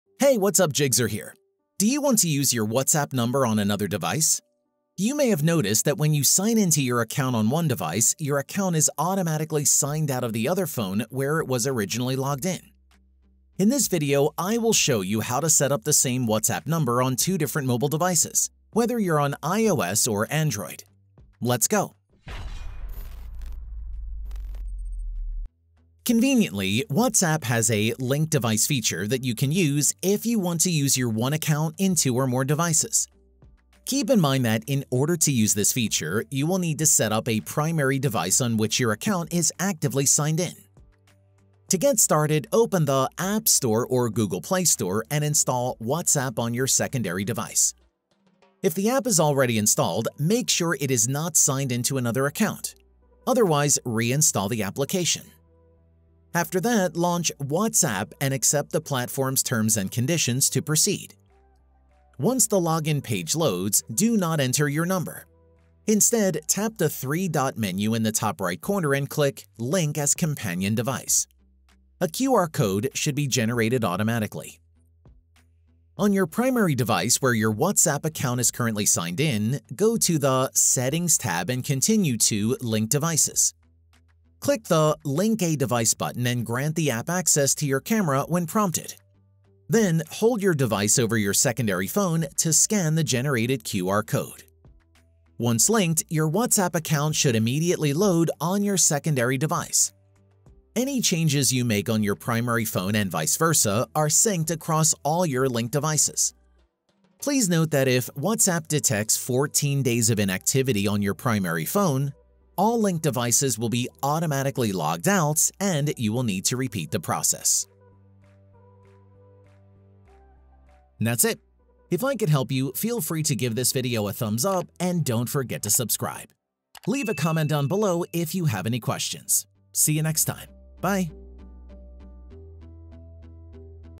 How to Setup same WhatsApp Account on two iPhones! [2024]
Have you ever noticed that when you try to sign in to your WhatsApp account on a different device, you get logged out? Well, it’s a good thing that you can actually use the same WhatsApp account on two different iPhones even if the platform doesn’t necessarily have the account switching feature. In this video, I will show you how to set this up on both of your iPhones. Yours Jigxor

Video: 653